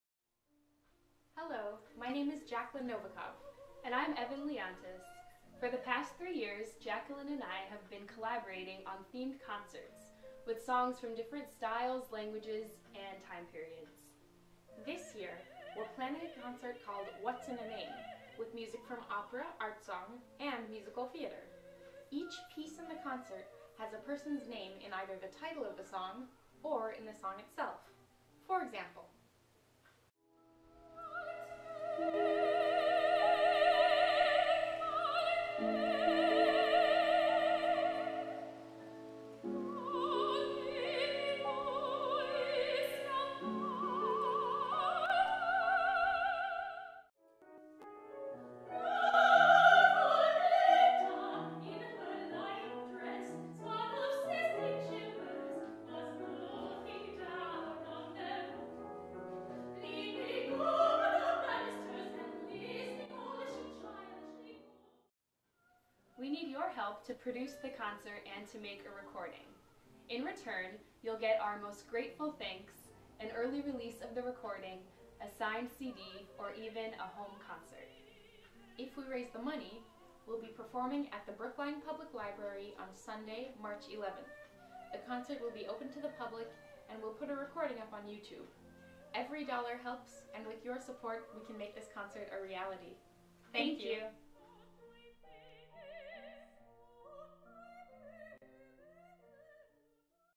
What's in a Name - Kickstarter Video
We're putting together a free classical concert in which every song is about a character whose name appears in the title or song text.